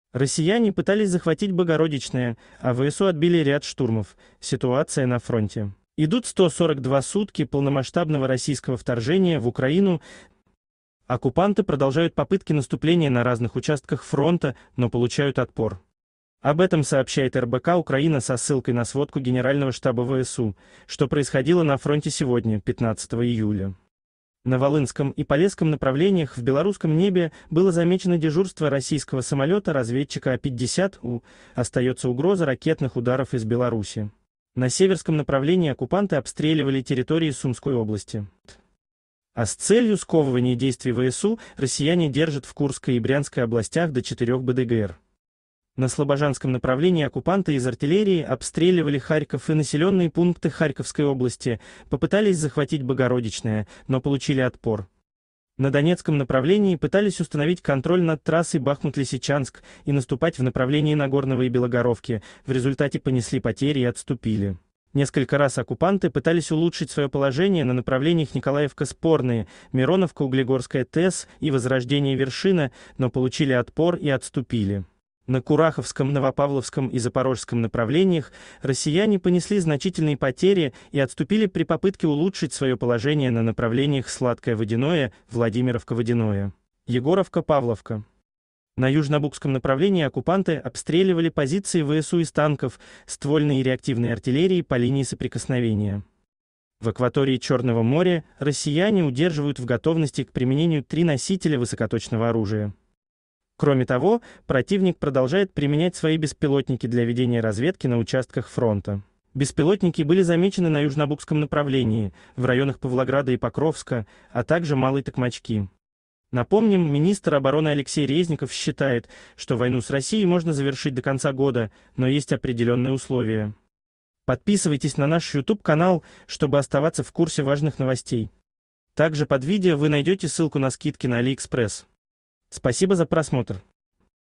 Россияне пытались захватить Богородичное, а ВСУ отбили ряд штурмов: ситуация на фронте.
ГенштабВСУ ВойнаРоссиипротивУкраины Получи скидку на Алиэкспресс по этой ссылке до 70% + дополнительные скидочные купоны от продавцов. Идут 142 сутки полномасштабного российского вторжения в Украину. Оккупанты продолжают попытки наступления на разных участках фронта, но получают отпор. Об этом сообщает РБК-Украина со ссылкой на сводку Генерального штаба ВСУ. Что происходило на фронте сегодня, 15 июля:. на Волынском и Полесском направлениях в белорусском небе было замечено дежурство российского самолета-разведчика А-50У, остается угроза ракетных ударов из Беларуси;. на Северском направлении оккупанты обстреливали территории Сумской области, а с целью сковывания действий ВСУ россияне держат в Курской и Брянской областях до четырех БТГр;. на Слобожанском направлении оккупанты из артиллерии обстреливали Харьков и населенные пункты Х